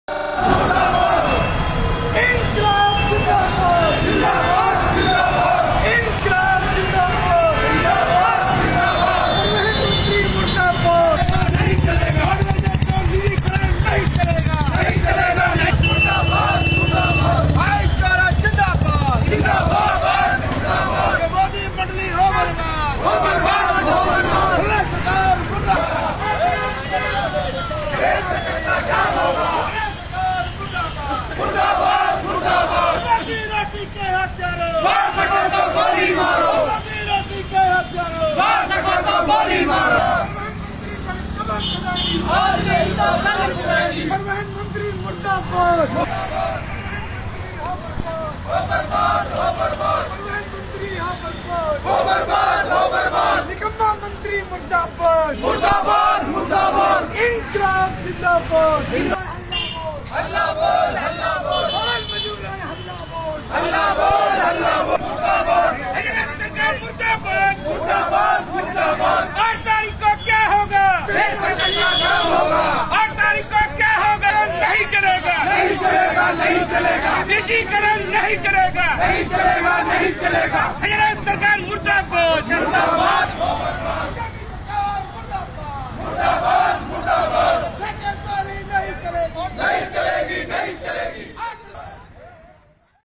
रोडवेज के कर्मचारियों ने आज बल्लमगढ़ की मार्केट में मशाल जुलूस निकालकर किया किलोमीटर स्कीम का विरोध
रोडवेज के कर्मचारियों ने आज बल्लमगढ़ की मार्केट में मशाल जुलूस निकालकर किया किलोमीटर स्कीम के किया विरोध प्रदर्शन। जिसमे फरीदाबाद नगर निगम के भी काफी संख्या में कर्मचारियों ने भाग लिया। और  हरियाणा सरकार के मुर्दाबाद के नारे लगाएं ।